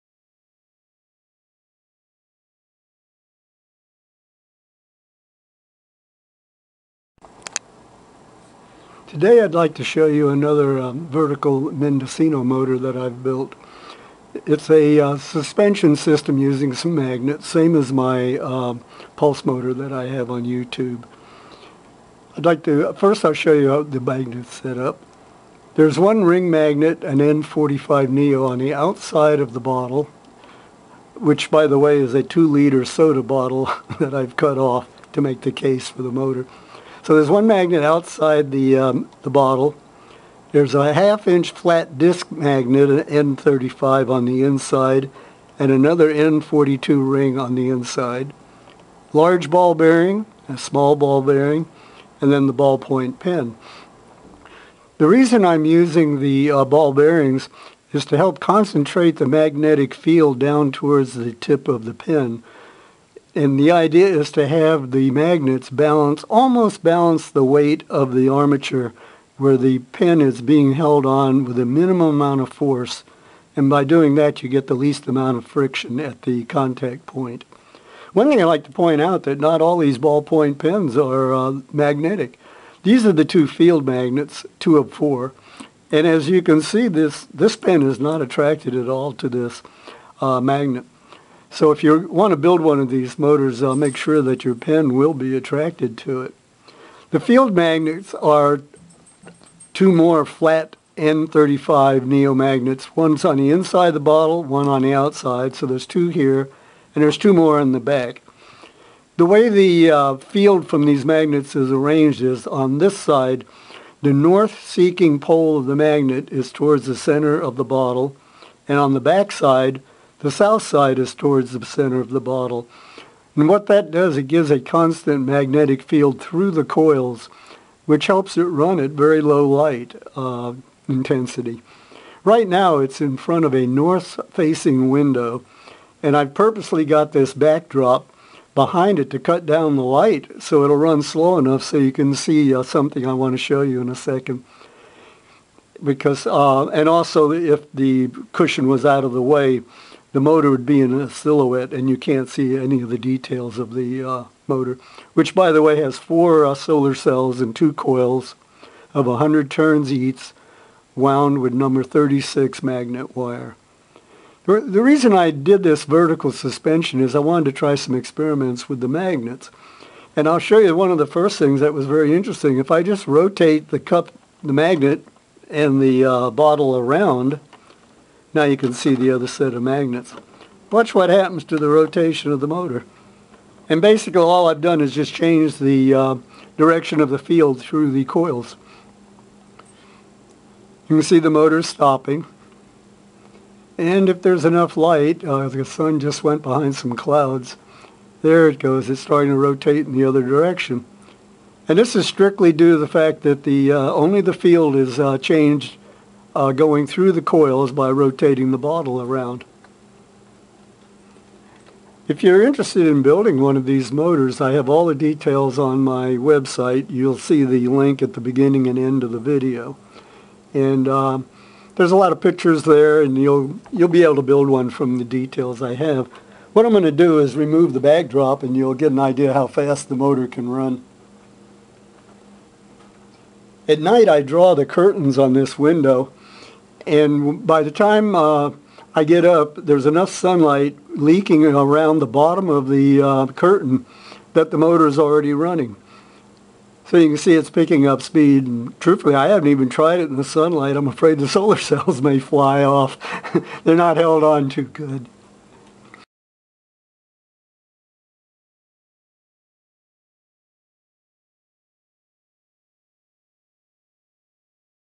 Mendocino motor in a bottle. A different way to build one.
This is a standard 4 solar cell, 2 coil Mendocino motor except for a few details.  It's vertically suspended and built into a 2L soda bottle.  Full details are given on three different magnet motors that you can build.
THE WEBSITE HAS MOVED TO   olrowdy.com   PLEASE UPDATE YOUR BOOKMARKS.  TNX